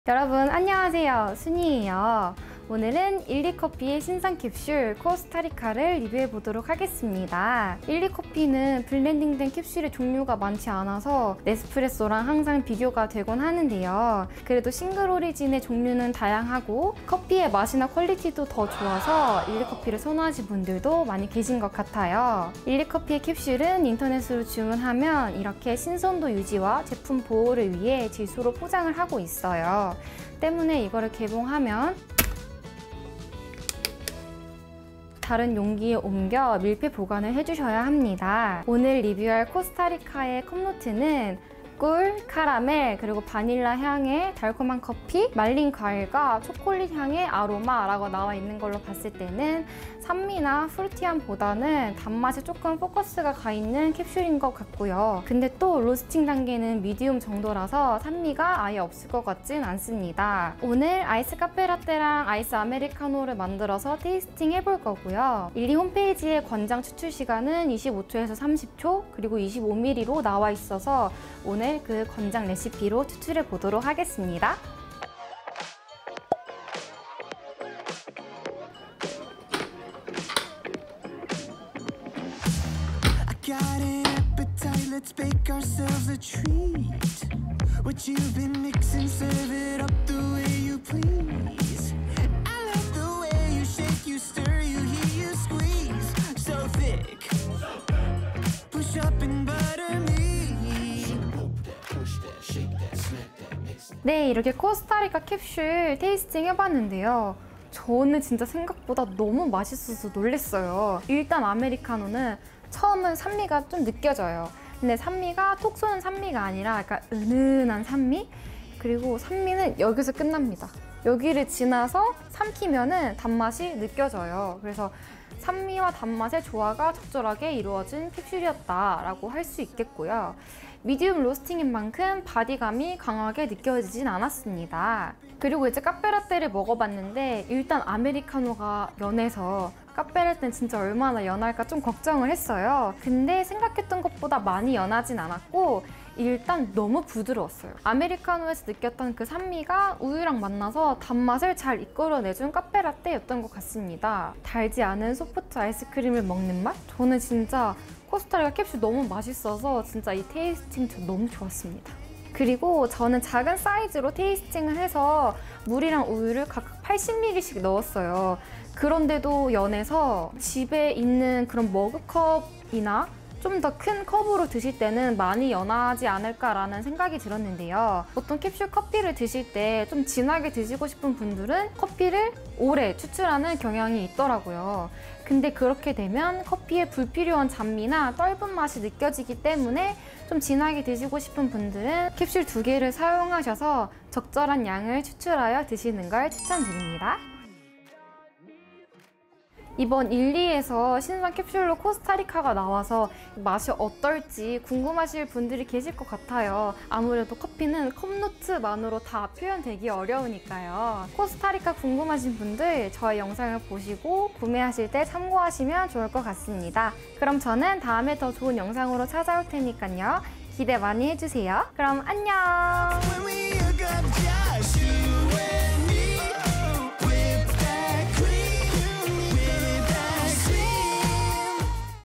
일리커피 신상 캡슐! 코스타리카 캡슐 리뷰☕️⎮illy coffee⎮Costa Rica Coffee
순이에요 :)

오늘은, 일리커피에서 새로 출시한
신상 캡슐! 코스타리카 캡슐을
테이스팅 해보았습니다 :)

기대했던 것보다 훨씬 맛있어서
영상 찍는 내내 기분이 좋았어요☺️

신상 캡슐이라 정보가 많이 없어서
구매할지말지 고민하시는 분들께
저의 영상이 조금이나마 도움이 되길 바랍니다!

그럼 다음에 봐요오
안뇽!💙